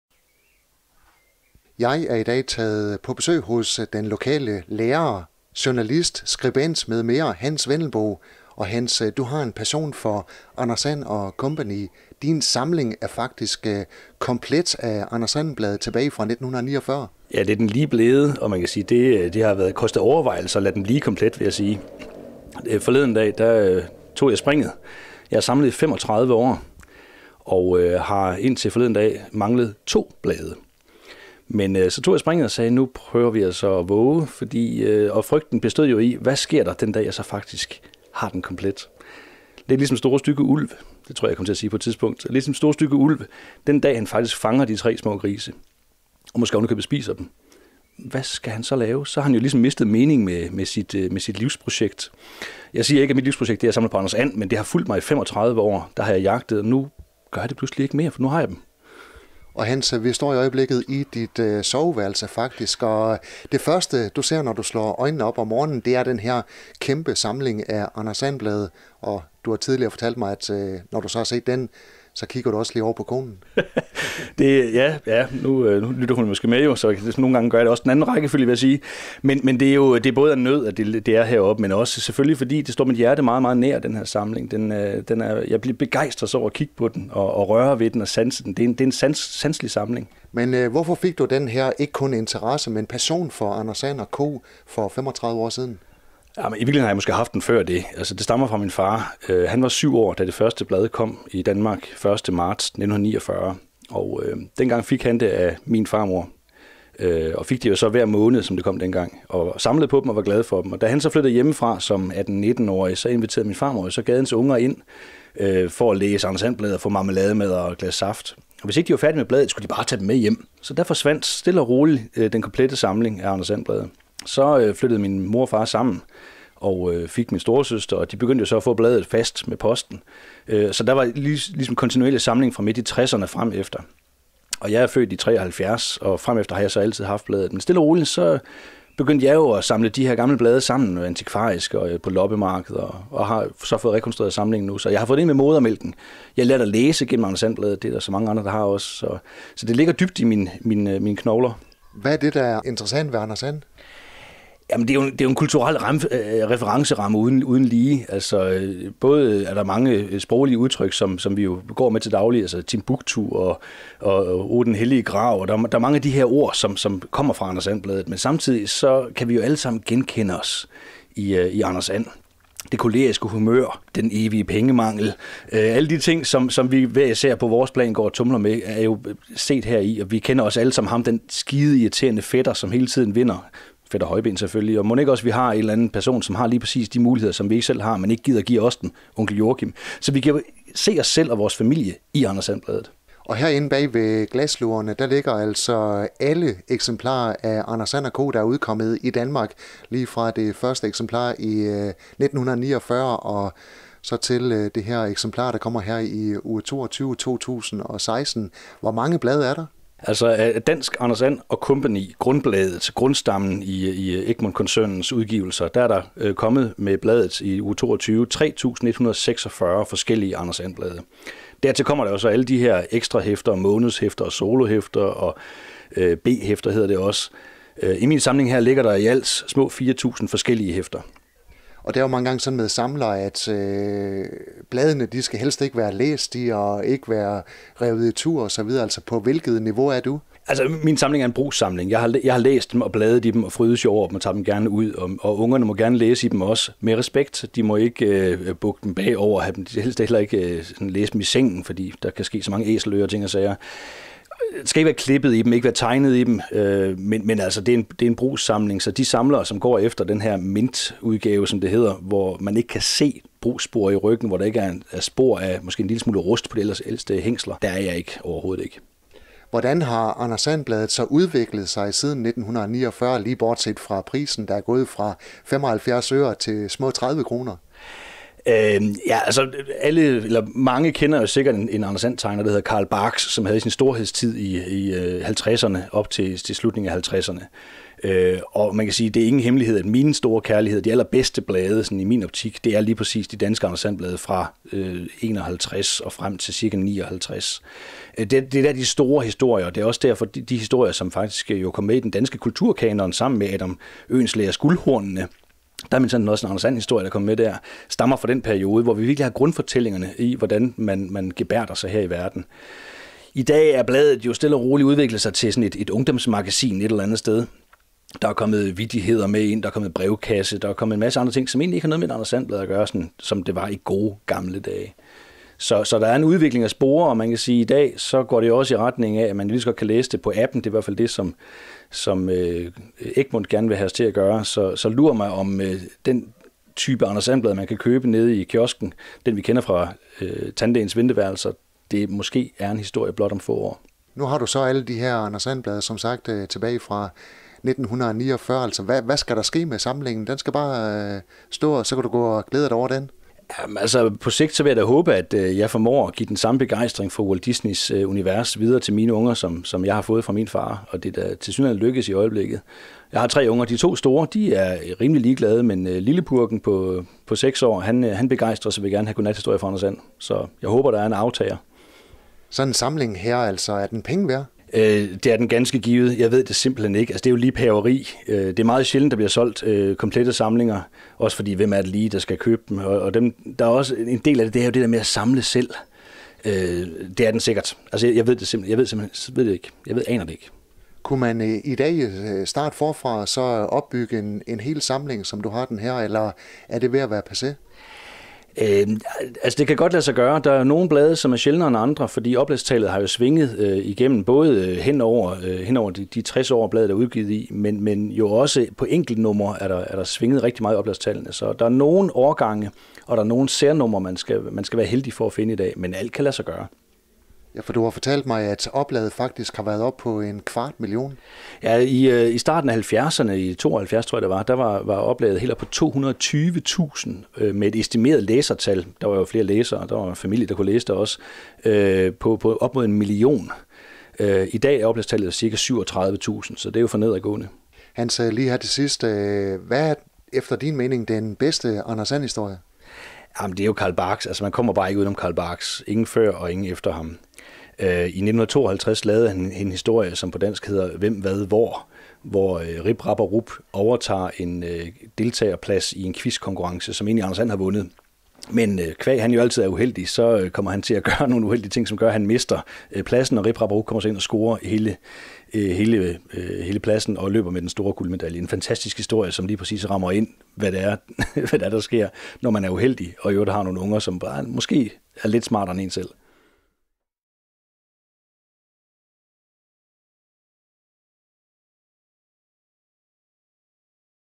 Interview Hans Wendelboe
Hans Wendelboe har en passion for Anders And & Co. De seneste 35 år har han samlet på Anders And blade og forleden dag blev samlingen komplet, så han nu er indehaver af 3146 Anders And blade tilbage fra 1949 og frem til i dag